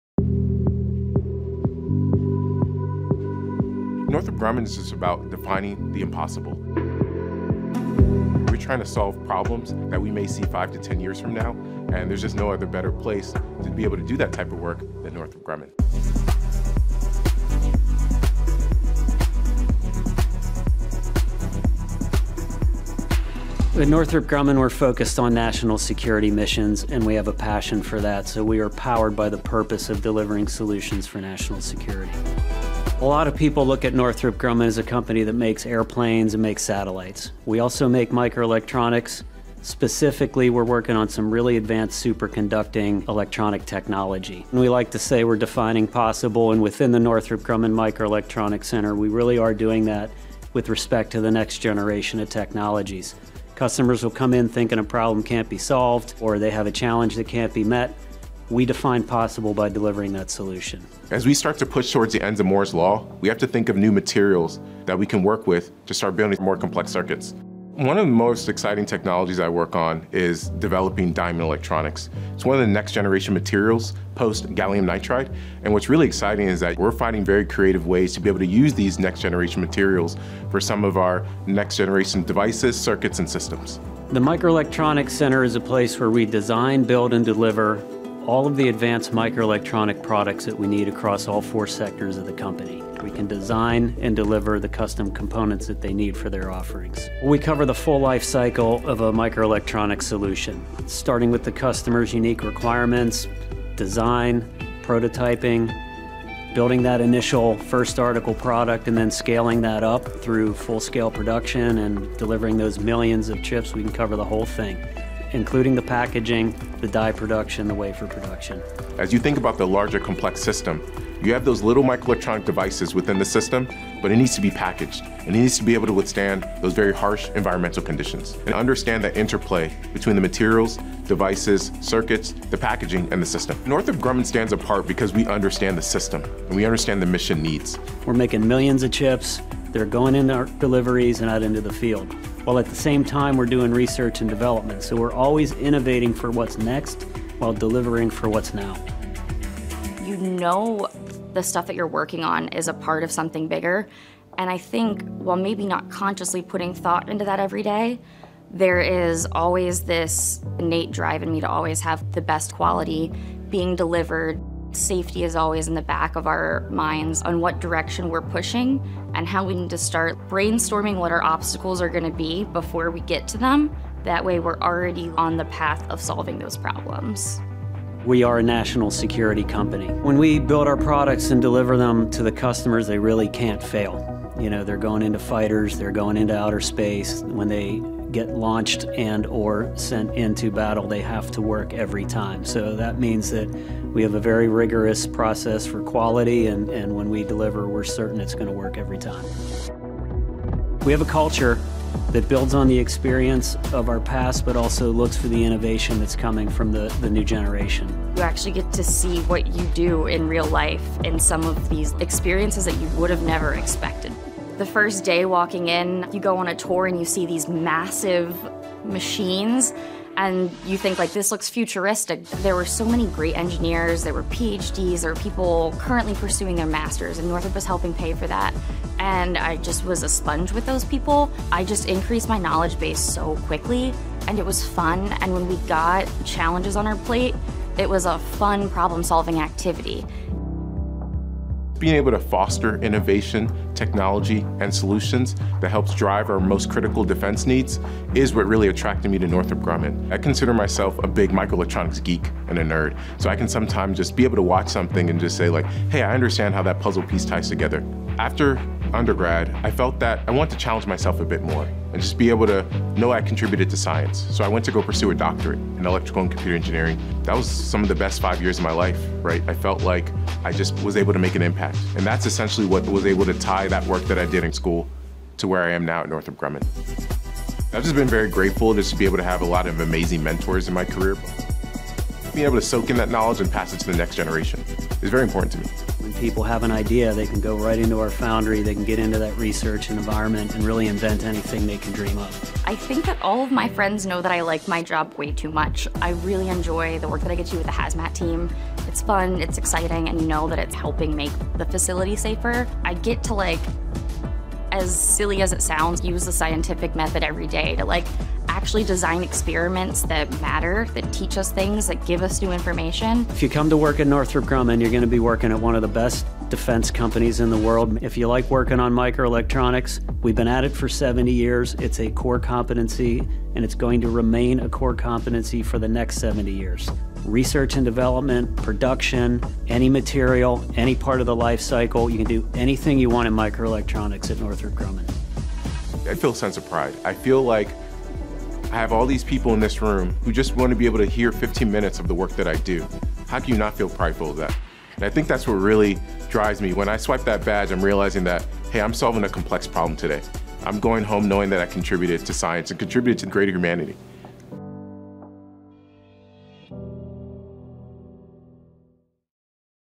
Silicon Guardians: Protecting America's Microelectronics - Northrop Grumman
Northrop Grumman stands at the forefront of advanced microelectronics innovation, powering both defense and commercial applications with groundbreaking precision. With over 70 years of government-trusted expertise, Northrop Grumman designs and develops specialized microelectronics for the Department of Defense, U.S. government entities, and industry partners. Its state-of-the-art facilities on American soil, supported by a highly skilled workforce, produce millions of chips annually, ensuring unmatched quality and reliability. Leveraging trusted foundries and pioneering onshore advanced packaging techniques, Northrop Grumman delivers dual-use solutions that meet the rigorous demands of national security and commercial markets. The company's commitment to excellence and continuous innovation has cemented its role as a vital contributor to modern defense and global security. By combining a rich legacy of precision engineering with cutting-edge technology, Northrop Grumman continues to shape the future of microelectronics in a rapidly evolving technological landscape.